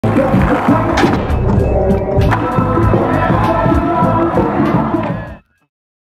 Paul Kruszewski Bigspin
Paul Kruszewski with the gnar at Hemming Plaza for Go Skate Day 2015.